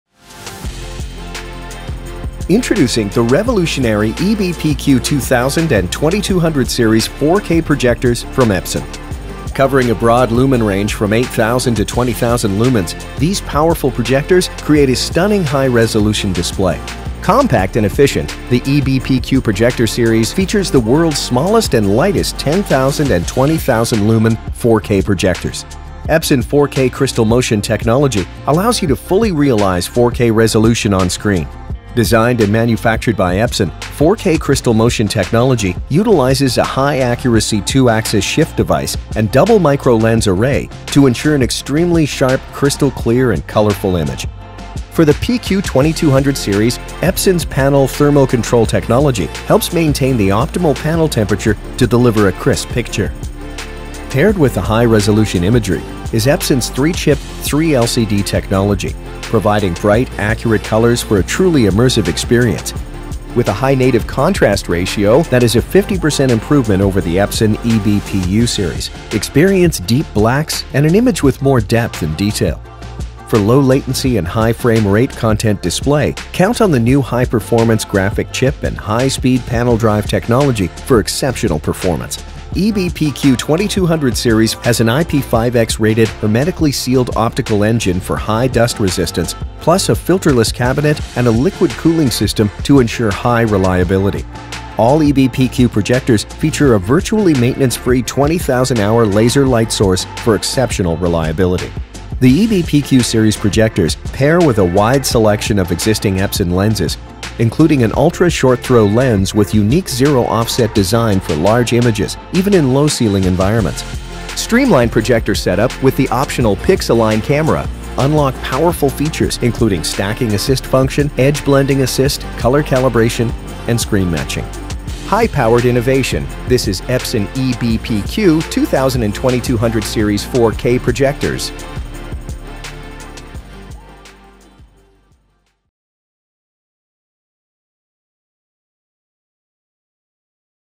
EB-PQ Series | Next-Level Large Venue Projectors
Produce captivating visuals in extraordinary detail with Epson’s EB-PQ Series of large venue 4K projectors. Delivering an impressive lumen range that cuts through ambient light, these projectors transform spaces with ultra bright images in virtually any environment. Compatible with a wide selection of Epson lenses, the EB-PQ Series excels in challenging installation spaces, including low-ceiling environments. And experience striking visuals featuring deep blacks and intricate image details thanks to a high native contrast ratio.